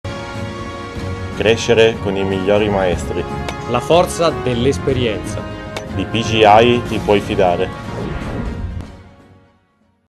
PGAI - GOLF TV COMMERCIAL - FULL VERSIONE 10" - 2011 - Sogg.1
Spot PGAI girato al ROYAL PARK GOLF CLUB - I ROVERI  dall'Agenzia Parking